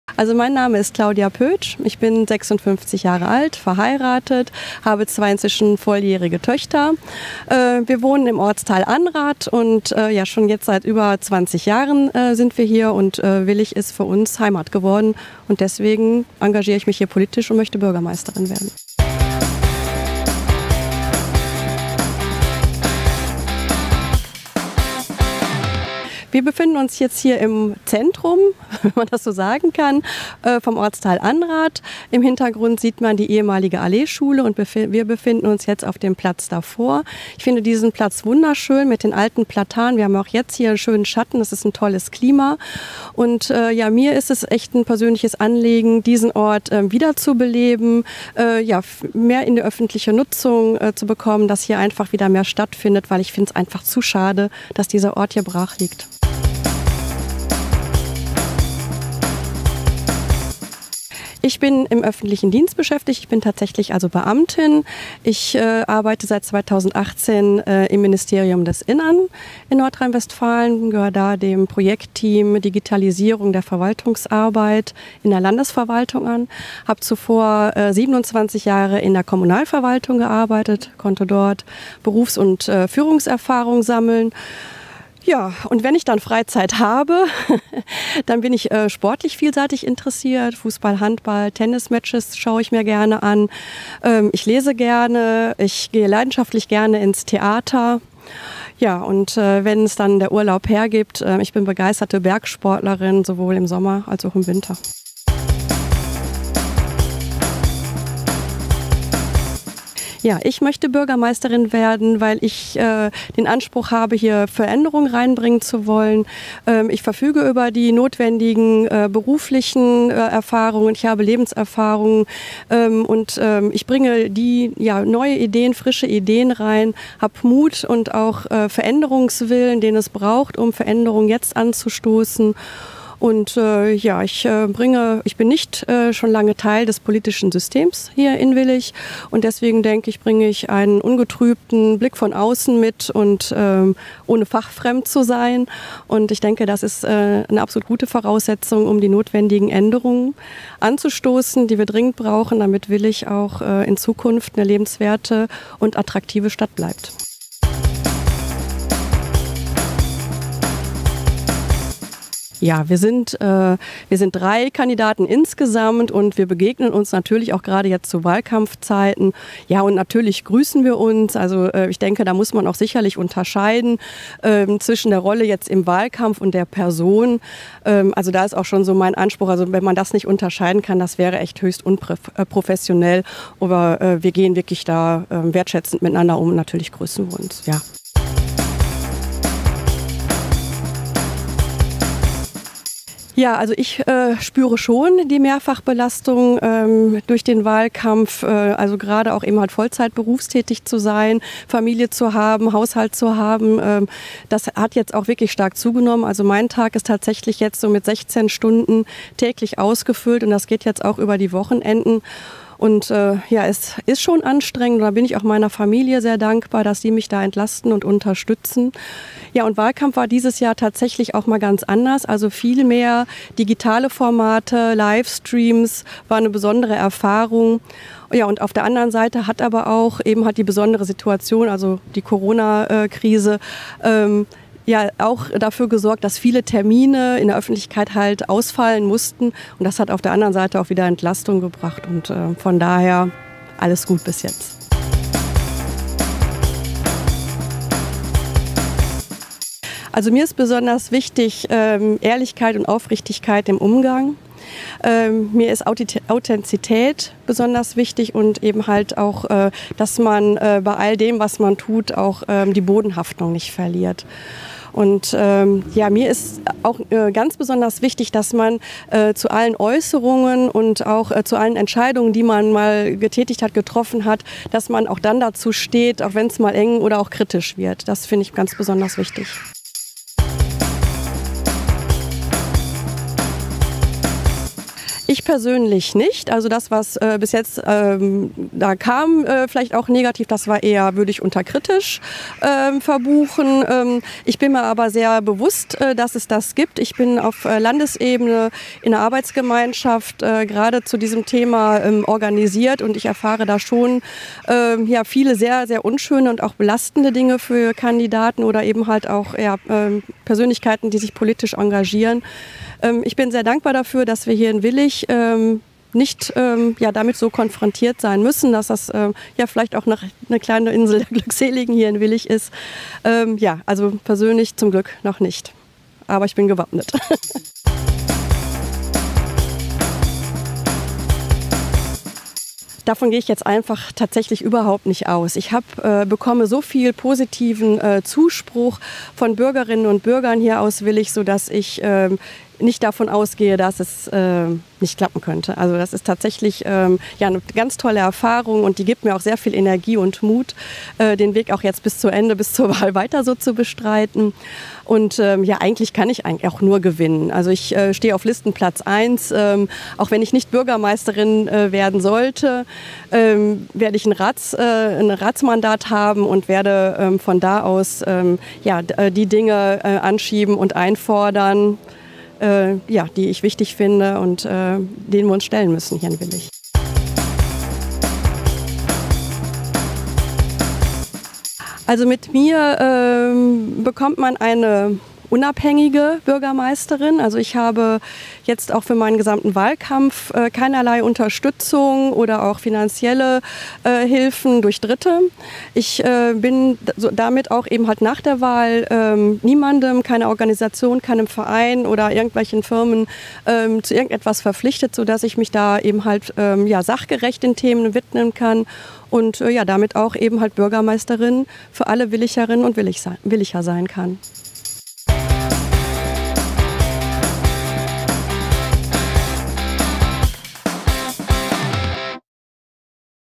Wahlcheck Niederrhein: Claudia Poetsch (Willich)
Am 13. September ist Kommunalwahl. Claudia Poetsch tritt für die Grünen in Willich als Bürgermeisterkandidatin an.